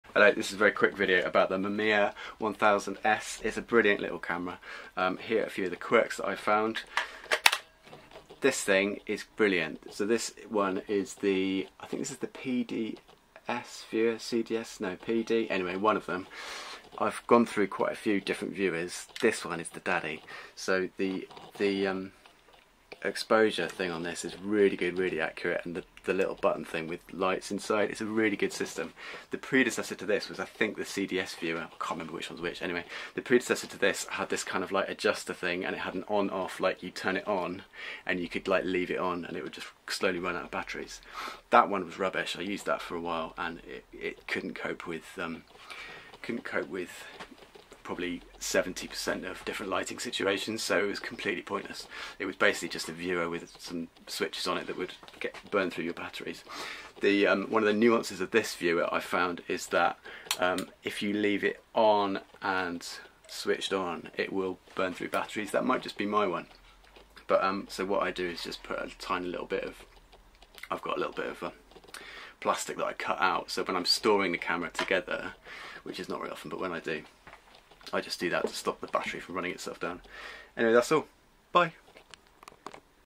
PD ... CDS ... the Mamiya 645 VIEWERS! Which one to use ... the PD, the CDS or the what?!
The Mamiya 645 1000 and 1000s Medium Format camera viewers - but which is best?!?!!